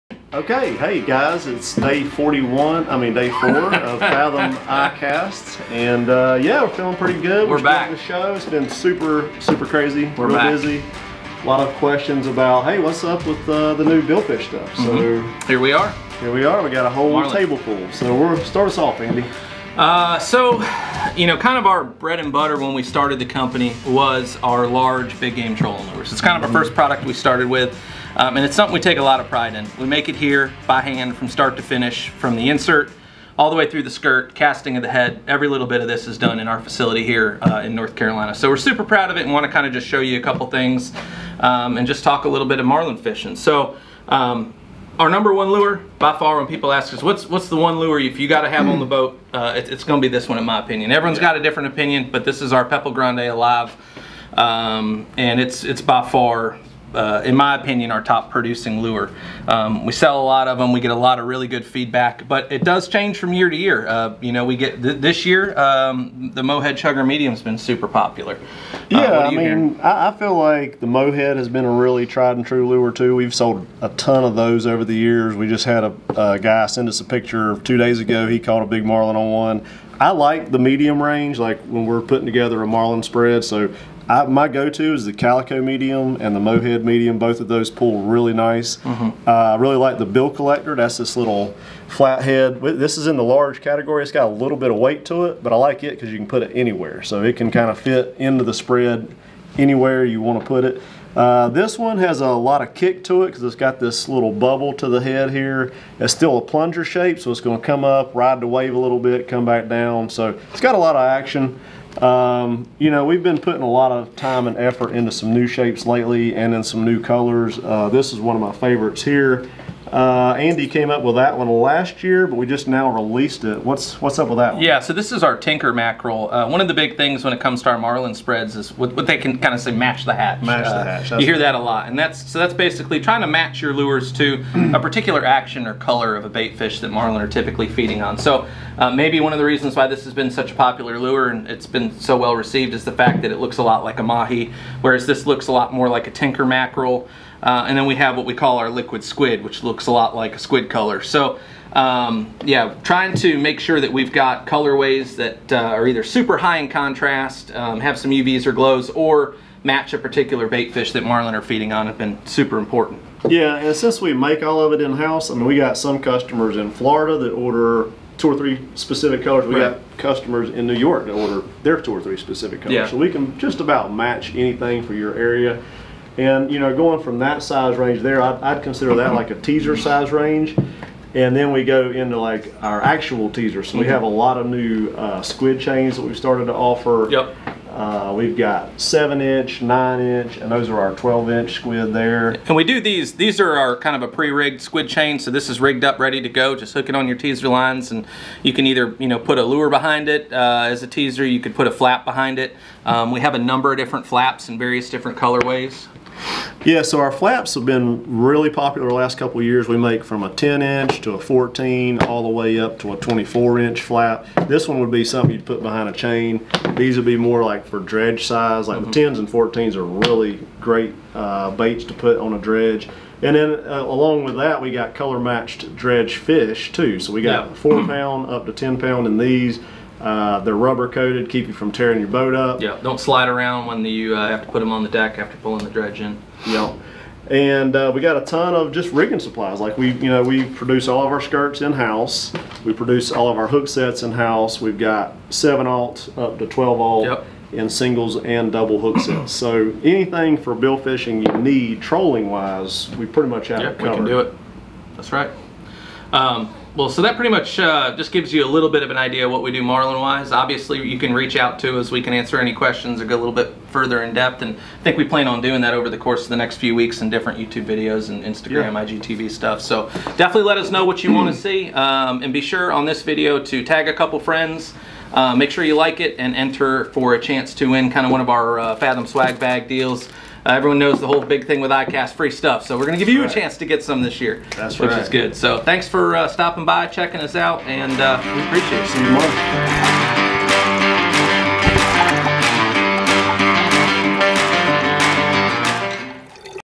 FathomCast 2020 Day 4: Marlin lures - ICAST 2020 we miss you! - FATHOM OFFSHORE
Our Large Big Game trolling lures is really how it all started for us at Fathom Offshore.

We take pride in making all our lures and skirts by hand, from start to finish, in our Wilmington, NC facility.

In this video we will show you are top producing lures world-wide like the Pepal Grande Large in Liquid Dolphin, Mo Head Chugger Medium and all new Tinker Mackerel.

Since we make everything in house, we can really fine tune and customize our lures as we get anglers feedback and fine tune our designs.

We touch on teasers and all the different options of squids and flaps, dredges and squid chains, hooksets and more!

Let us know if you have any more specific questions about our products or how we can help you pick the right products for your fishing spread.

We are planning to continue to produce these short style videos so your input is helpful in determining what kinds of questions we should address.